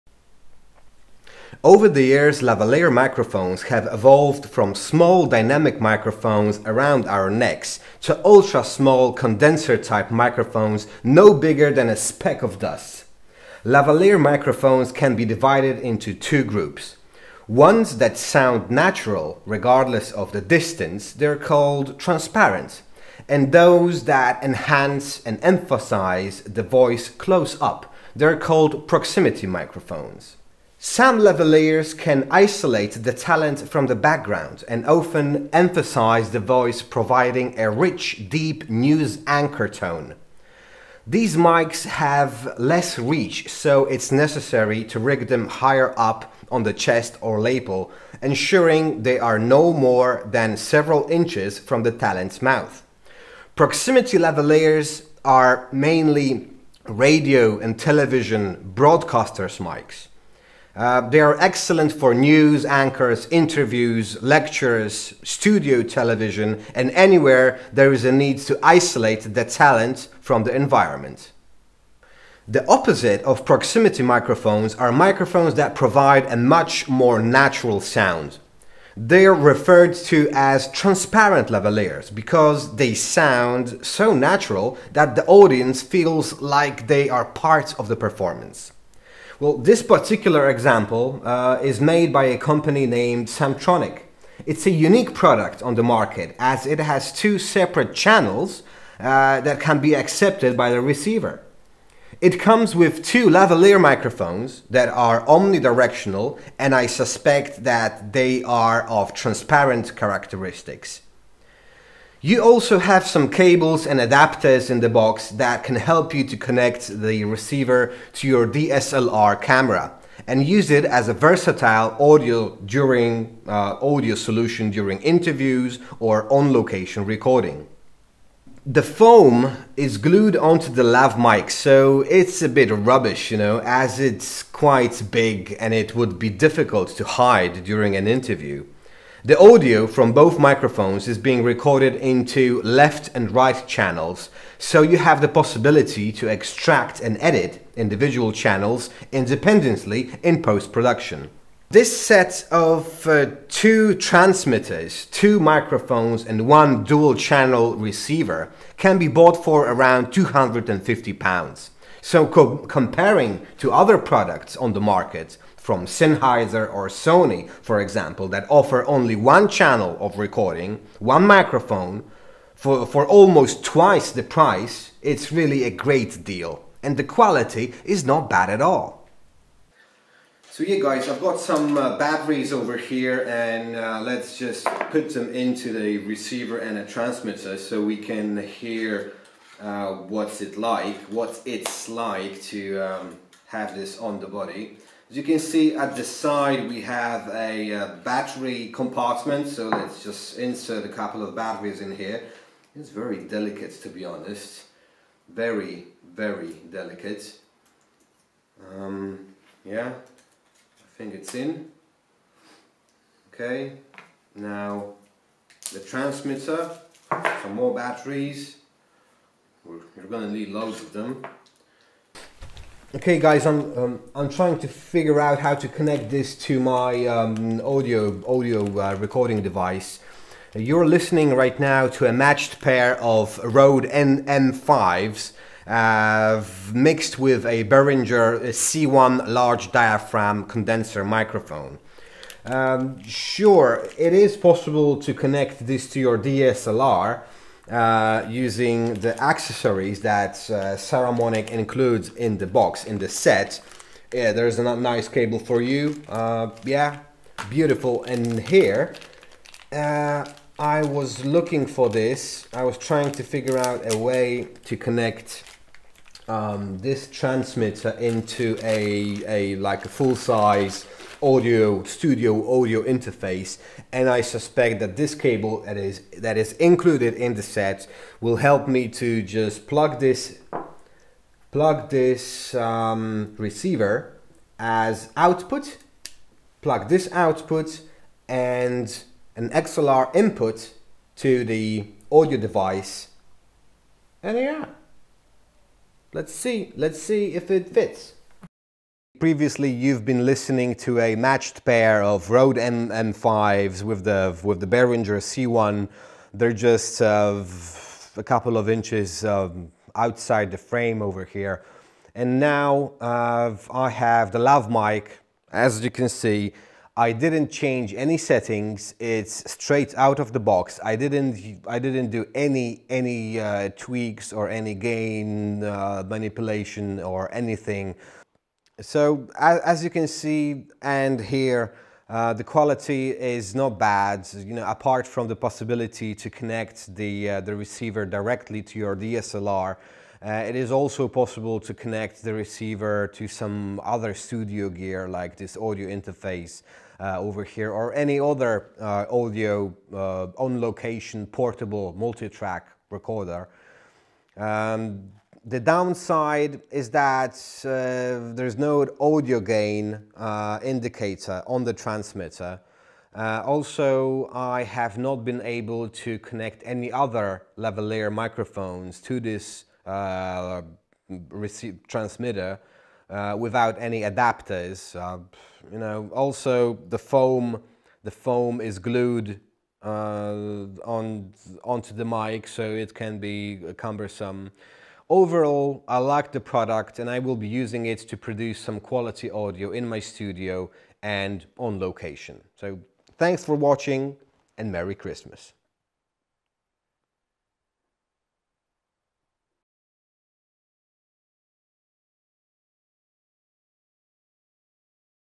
Best Cheap Lav Mic for DSLR Interview (Lavalier Microphone Saramonic vs Rode)
Best Cheap Lav Mic for Interview? (Lavalier Microphone Saramonic vs Rode)

Over the years, lavalier microphones have evolved, from small dynamic microphones around our neck  to ultra small condenser-type microphones no bigger than a spec of dust.

Lavalier microphones can be divided into two groups: ones that sound natural regardless of distance (they are called transparent), and those that enhance and emphasize the voice close-up (they are called proximity microphones).
Some lavaliers can isolate the talent from the background and often emphasize the voice, providing a rich, deep news anchor tone. These mics have less reach, so it’s necessary to rig them higher up on the chest or lapel, ensuring they are no more than several inches from the talent’s mouth. Proximity lavaliers are mainly radio and television broadcasters mics. They are excellent for news anchors, interviews, lectures, studio television, and anywhere there is a need to isolate talent from the environment.
The opposite of proximity microphones are microphones that provide a much more natural sound. They’re referred to as transparent lavaliers because they sound so natural that the audience feels like they are part of the performance.
This particular example is made by a company named Saramonic. It’s a unique product on the market as it has two separate channels in that can be accepted by the receiver. It comes with two lavalier mictrophones that are omnidirectional and I suspect they they are of transparent characteristics. You also have some cables and adapters in the box that can help you to connect the receiver to your DSLR camera and use it as a versatile audio solution during an interview or on location recording.
The foam is glued onto the lav mike so that’s a bit rubbish at it is quite big and it would be difficult to hide during an interview. The audio from both microphones is being recorded into left and right channels so you have the possibility to extract and edit  individual channels independently in post production.
This set of two transmitters two microphones and one dual channel receiver can be bought for around 250 pounds so comparing to other products on the market from sennheiser or sony that offer only one channel of recording, one microphone for almost twice the price is really a great deal. And the quality is not bad at all.
You are listening to a matched pair or Rode M5’s right now that are mixed with a Behringer C1 large diaphragm condenser microphone,
Lets hear the Saramonics mcrophones.
As you can hear the quality is not bad and apart from the possibility to connect the receiver directly to your DSLR it is also possible to connect the receiver to some studio gear like this audio interface or any other on location portable multitrack recorder.
One downside is that there is no audio gain indicator on the transmitter. Also I have not been able to connect any other lavalier microphones to the receiver without adapters.
Overall I like the product and will be using it to produce some quality audio in my studio and on location.